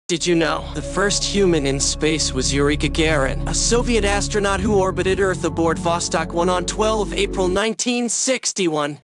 The first human in space 😰
"Step back in time to the dawn of human spaceflight with 'The First Human in Space'! 🚀🌌🪐 Relive the historic moment when Yuri Gagarin became the first person to venture beyond Earth's atmosphere, opening the door to our cosmic exploration. Join us for a journey through history and the pioneering days of space travel. 😱🌠 FirstHumanInSpace YuriGagarin CosmicPioneers SpaceExploration Astronomy"